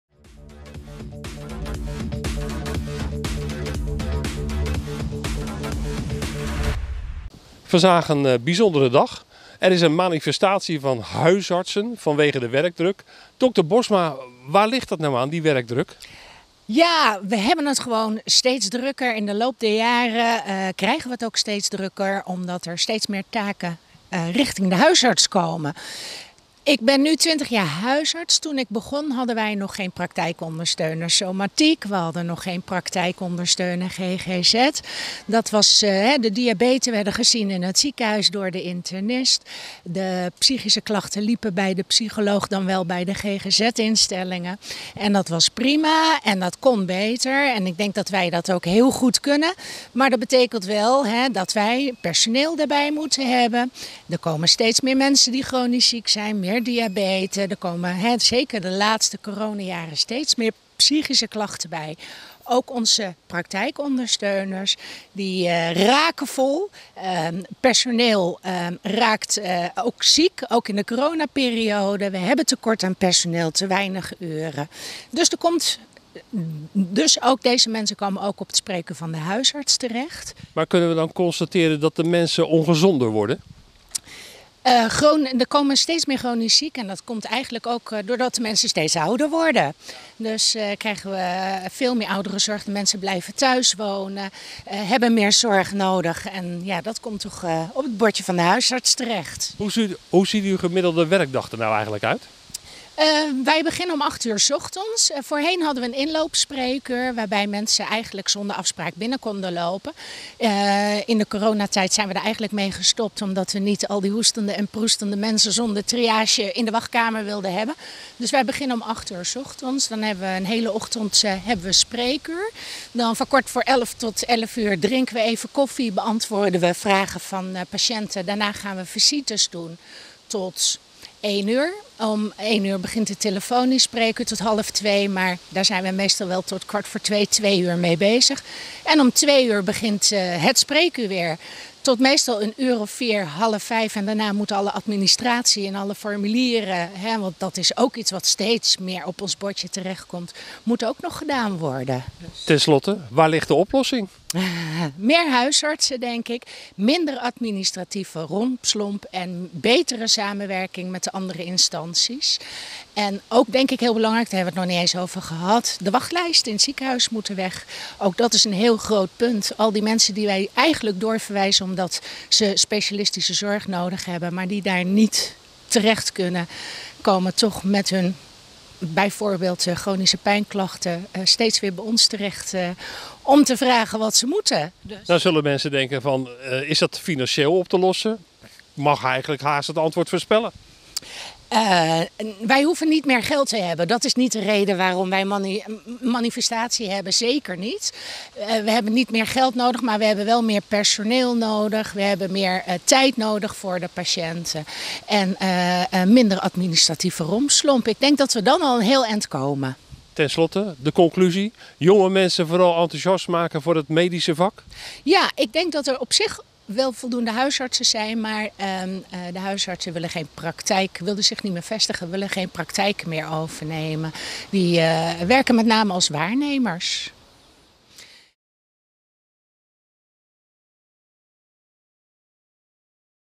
Huisarts Bosma over manifestatie huisartsen
Vandaag is er een landelijke manifestatie van huisartsen over de te grote werkdruk.
JW TV sprak met huisarts dr. Bosma van Huisartsengroep Kethel over deze werkdruk.